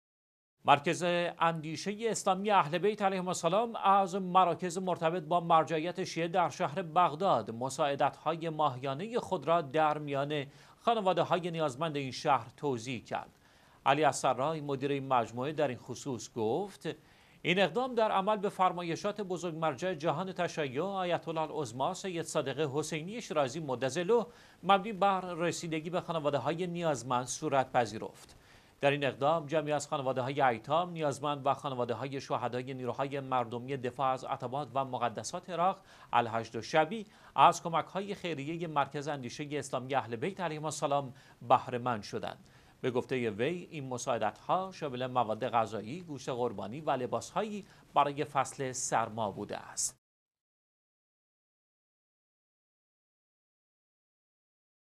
توزیع کمک‌های ماهیانه در میان خانواده‌های نیازمند شهر بغداد
مرکز اندیشه اسلامی اهل بیت علیهم السلام، از مراکز مرتبط با مرجعیت شیعه در شهر بغداد مساعدات ماهیانه خود را در میان خانواده ‌های نیازمند این شهر توزیع کرد.

علی السرای، مدیر این مجموعه در این خصوص گفت: این اقدام در عمل به فرمایشات بزرگ مرجع جهان تشیع آیت الله العظمی سید صادق حسینی شیرازی دام ظله مبنی بر رسیدگی به خانواده‌های نیازمند صورت پذیرفت. در این اقدام  جمعی از خانواده‌های ایتام، نیازمند و خانواده‌های شهدای نیروهای مردمی دفاع از عتبات و مقدسات عراق، الحشد الشعبی، از کمک های خیریه مرکز اندیشه اسلامی اهل بیت علیهم السلام بهره مند شدند.
به گفته وی این مساعدت ها شامل مواد غذایی، گوشت قربانی و لباس هایی برای فصل سرما بوده است.